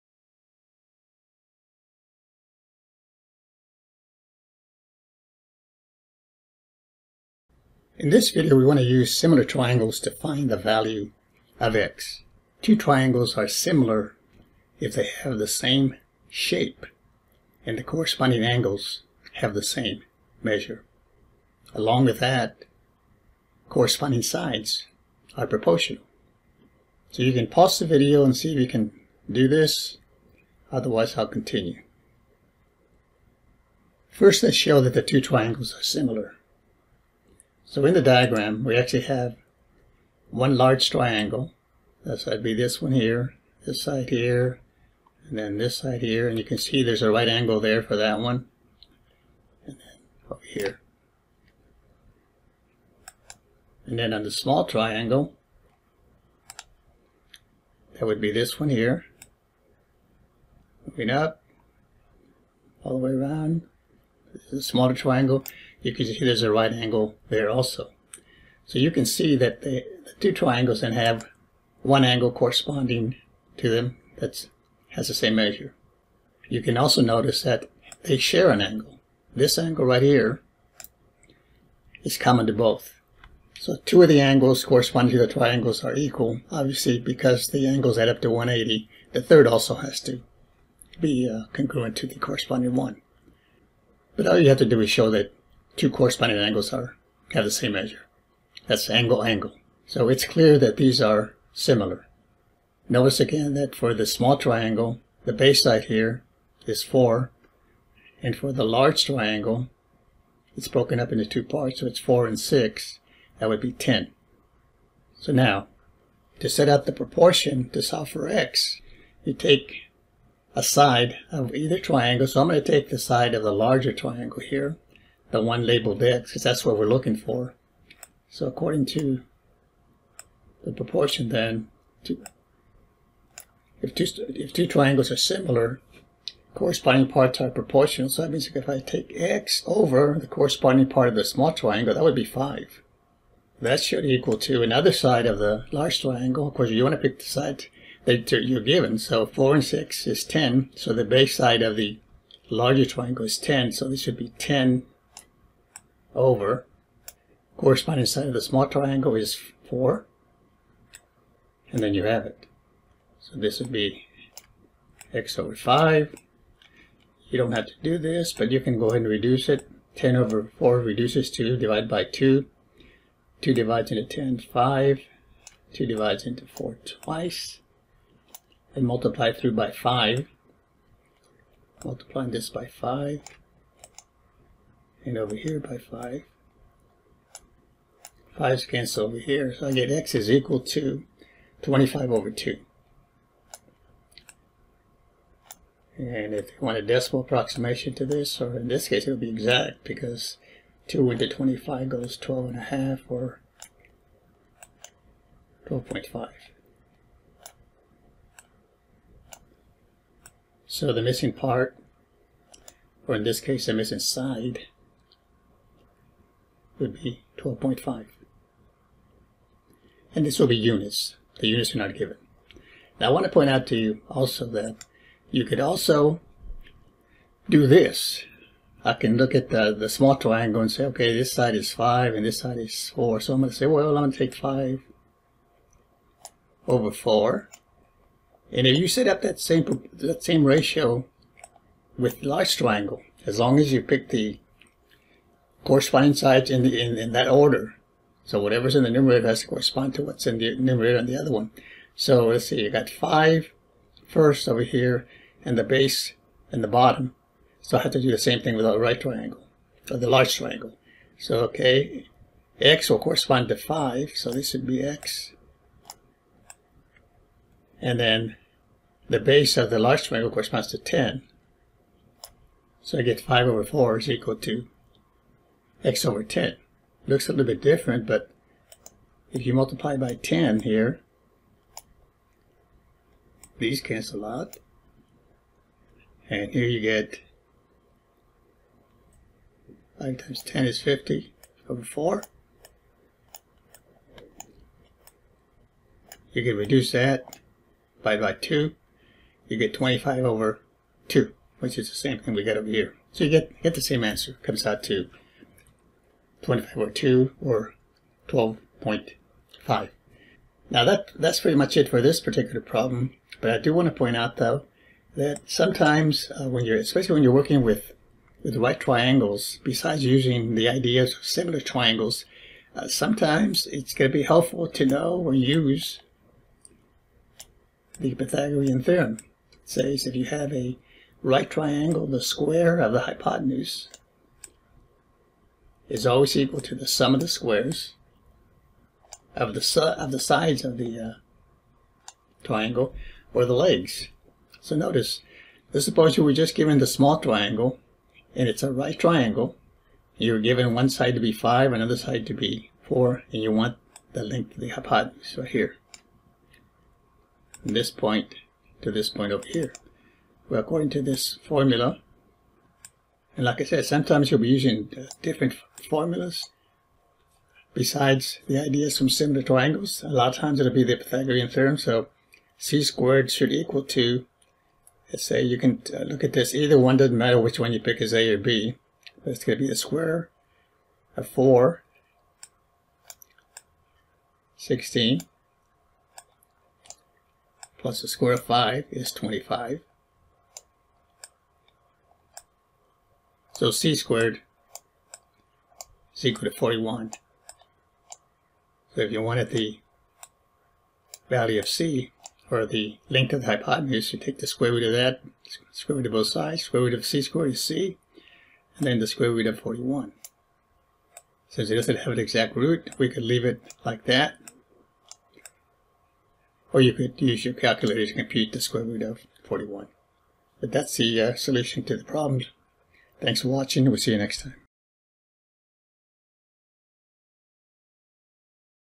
Use Similar Triangles to Find the Value of X
In this video, we use similar triangles to find a value for X.